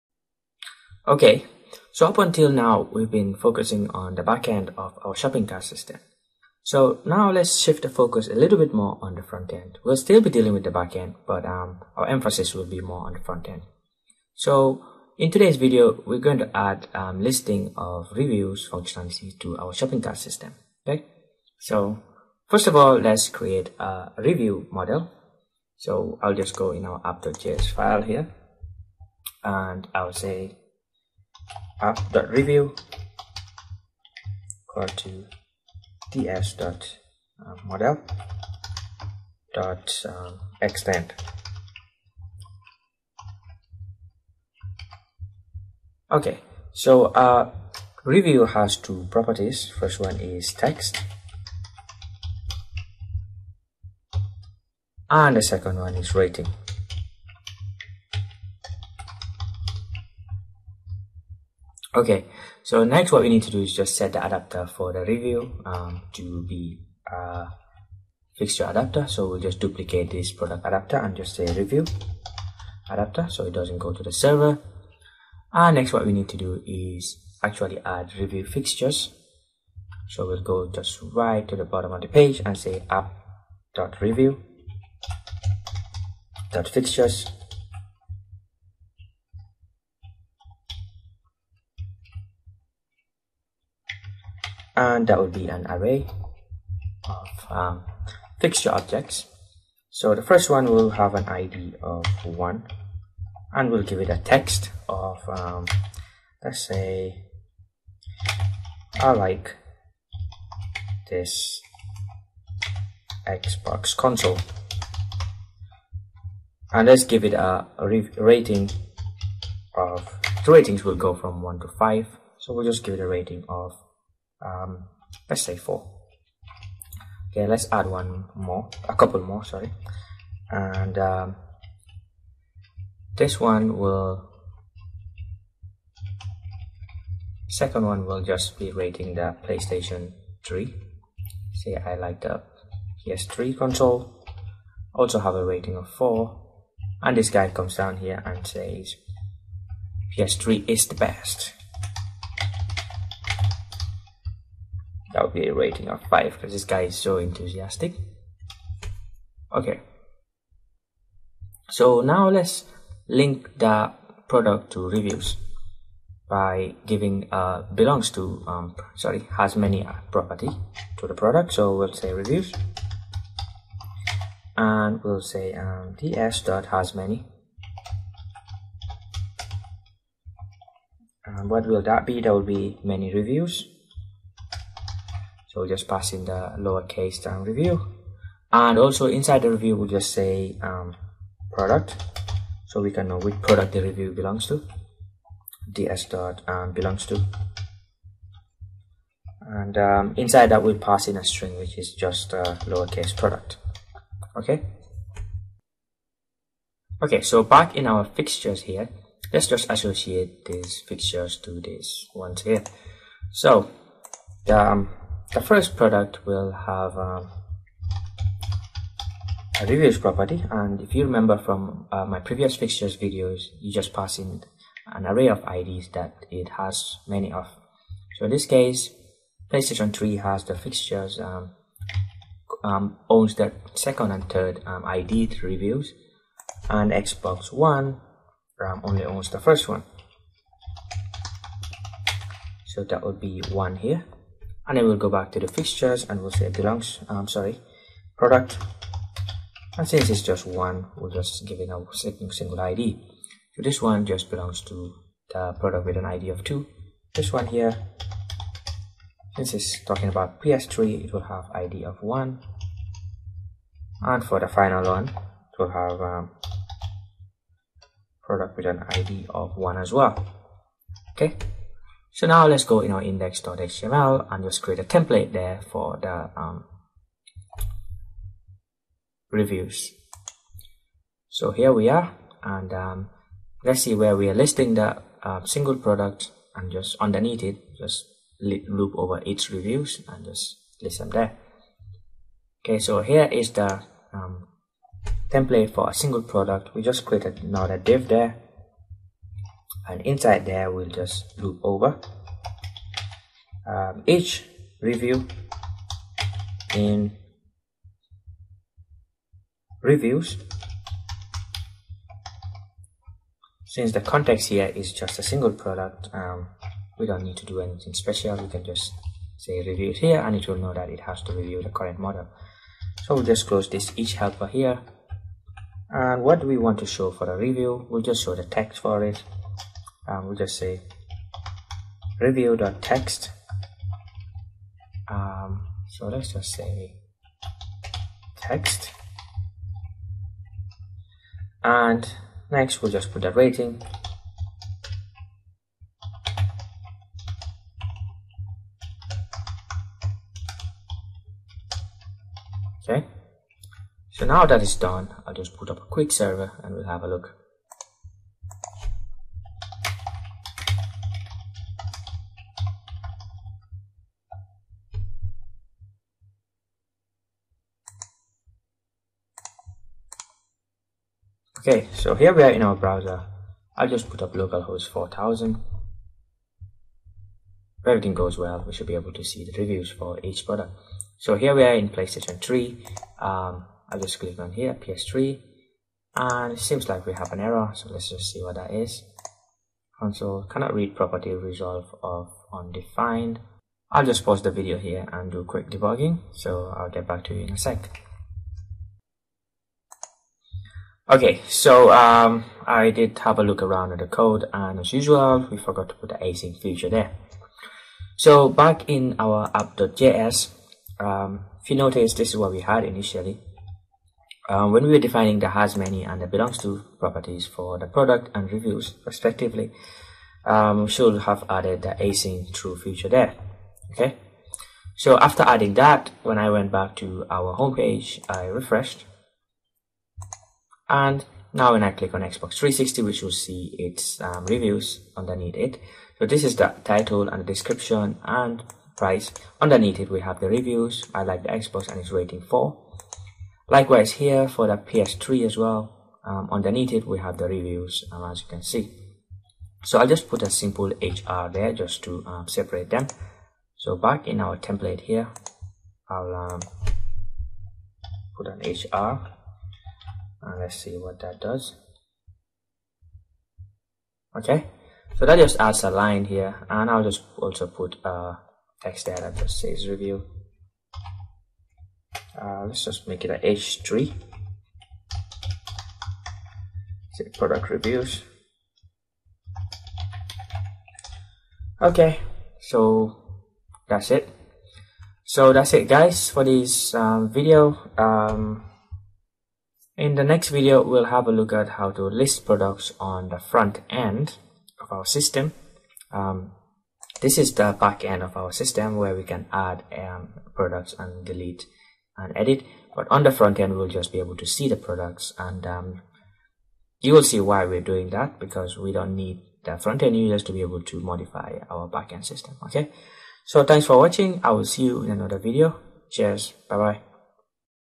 EmberJs Simple Shopping Cart System - 6 List Product Reviews
We finally get to add a reviewing system to our shopping cart. For this video, we'll create a review model, fixtures and list them under a product.